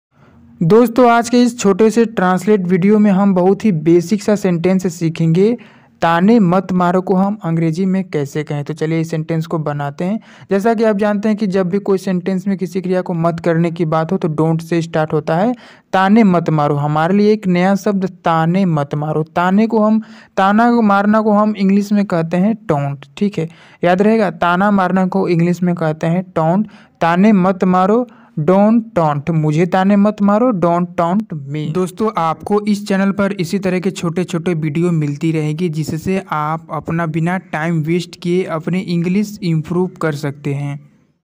tane mat maro ko english mein kya kahate hai // ताने मत मारो को इंग्लिश में कैसे कहें ? spoken
tane mat maro ko english mein kya kahate hai // ताने मत मारो को इंग्लिश में कैसे कहें ? spoken english

दोस्तों इस छोटे से वीडियो में हमने सीखा एक सेंटेंस को इंग्लिश में कैसे बोलते हैं सेंटेंस को ना केवल हमने सिर्फ सीखा बल्कि इसे समझा भी  ताकि आप इसी तरह के और भी सेंटेंस आसानी से बना सके तो इसी तरह की छोटे छोटे वीडियो बिना किसी बकवास के सीखने रहने के लिए चैनल पर नए है तो चैनल को सब्सक्राइब कर लेना